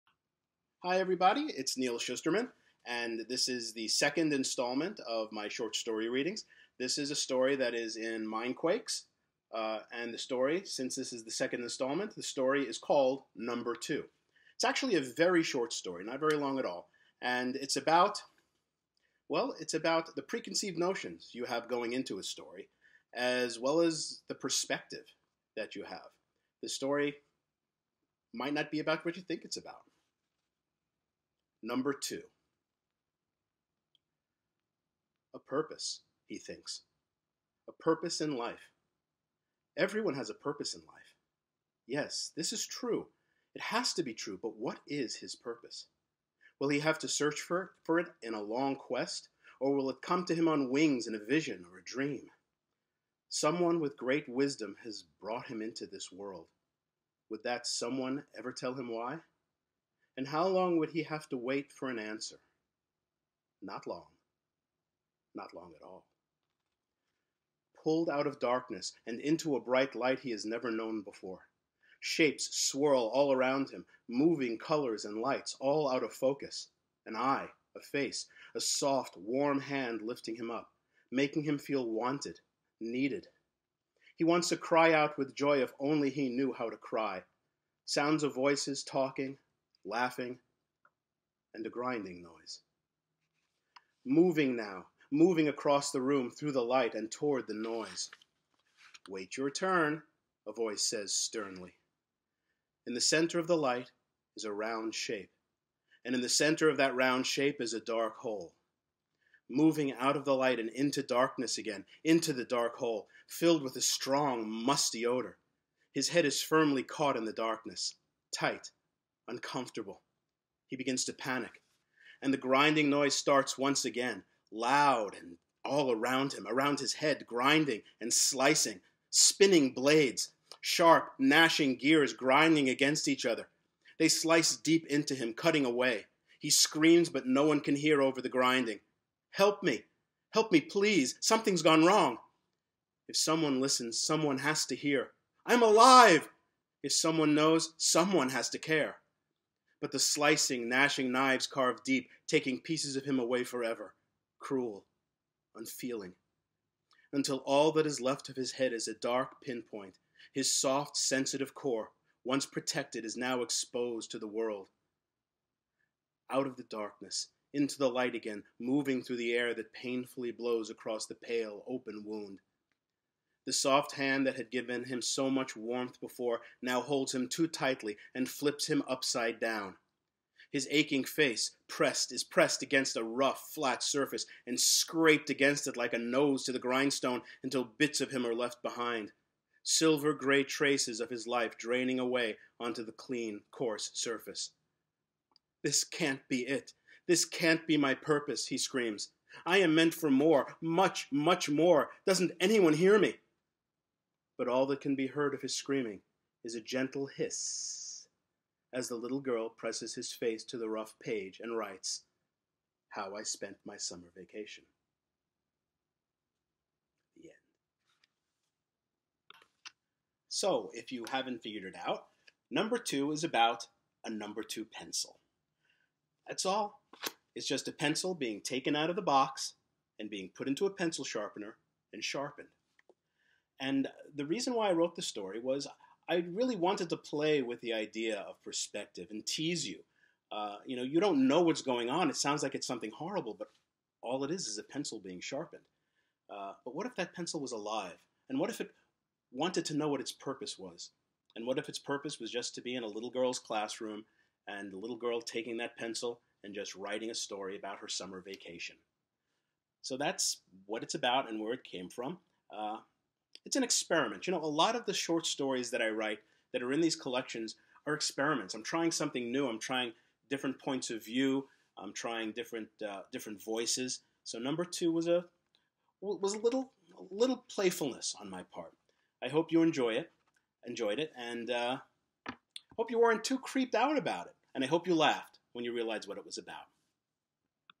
Number Two | Storyman Virtual Reading Series
Welcome to the Storyman Virtual Reading Series, in which I'm reading selections of my work for students, educators, librarians, and readers of all kinds around the world. Today's installment is "Number Two" from my short story collection MINDQUAKES.